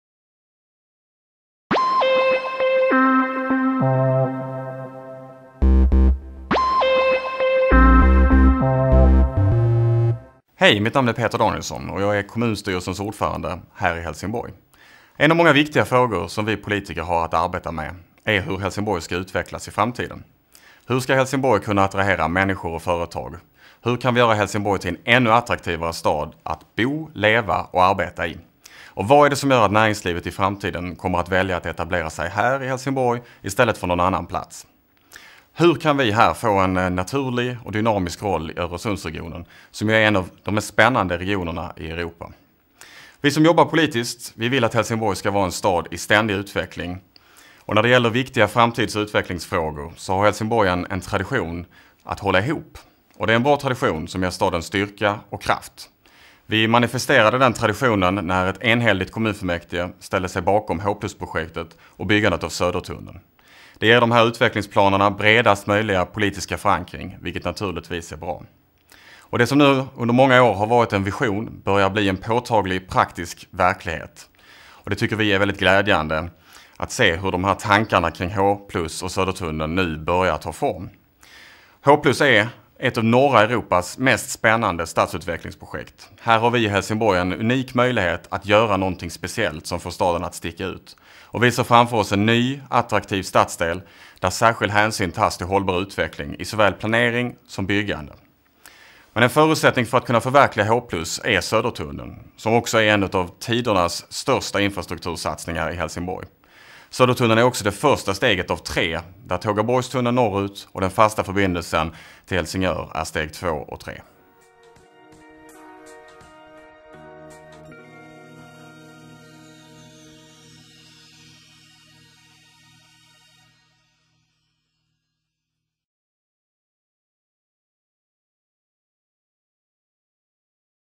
Film om det spännande stadsförnyelse projektet H+ i Helsingborg, Peter Danielsson
Film om det spännande stadsförnyelse projektet H+ i Helsingborg. Lyssna till kommunstyrelsens ordförande Peter Danielsson. Nedkortad version från 2009